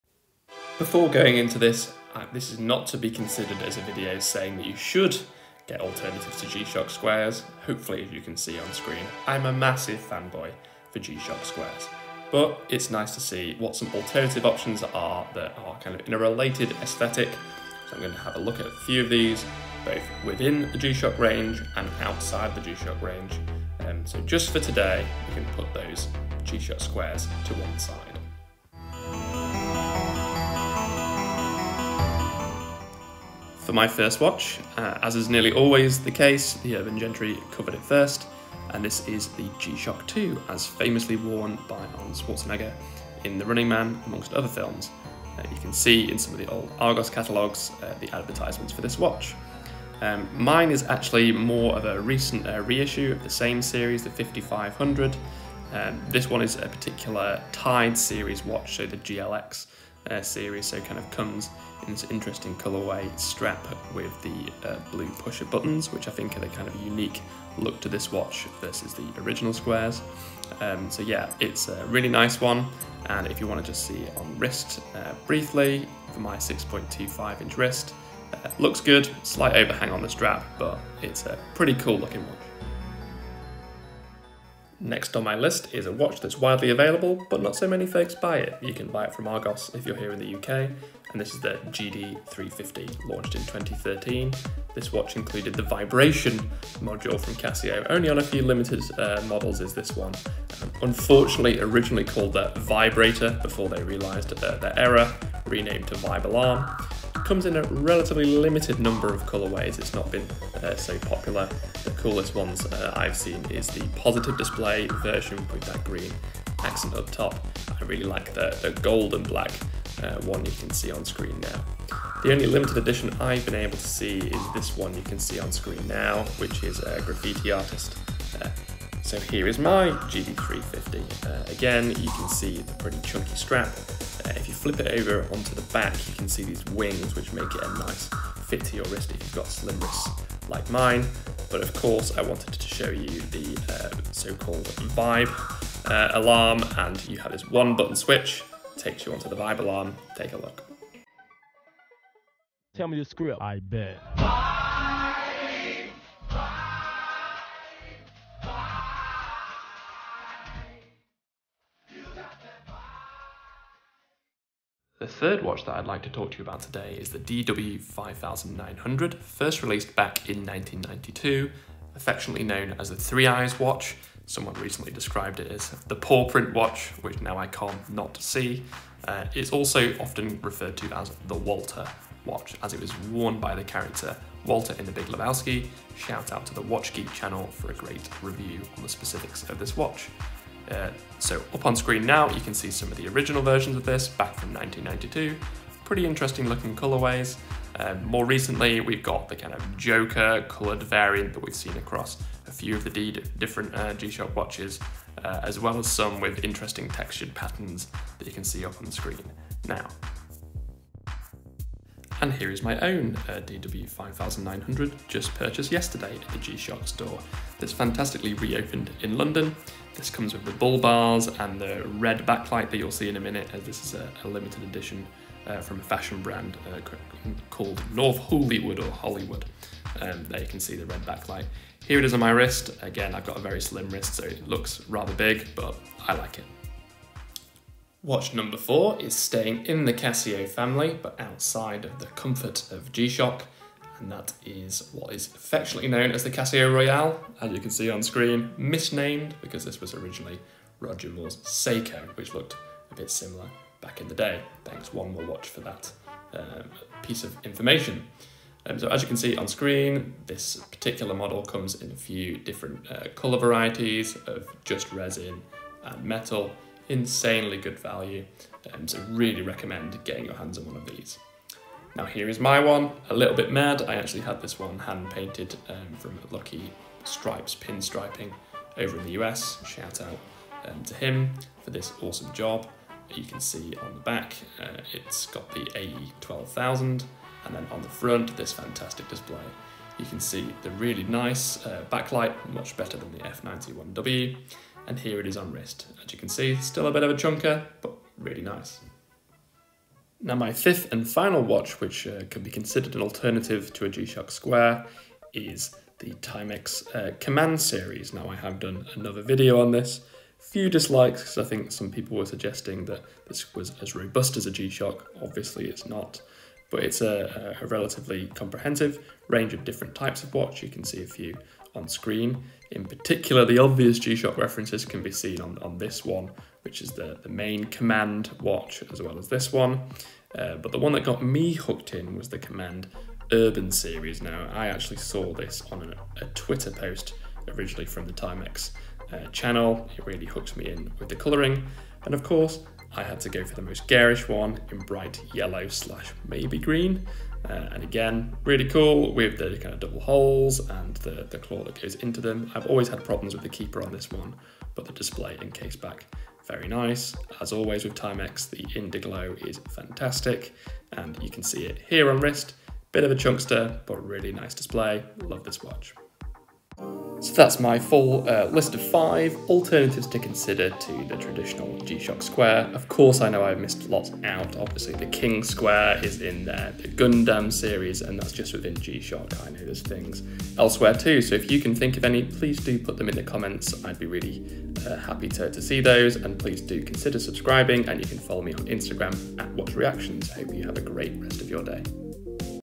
5 G-SHOCK SQUARE ALTERNATIVES - Some G-shock and non G-Shock options for those that love a Square
I love a digital watch and in particular the Casio G-Shock square with the 5600 series being the most classic and collected.  However, if you are looking for a related aesthetic but something a bit different i’ve got 5 suggestions for you from within the G-shock range, as well as some other brands to keep things interesting.  Liberally used throughout is the Shockbase website that you should consider visiting.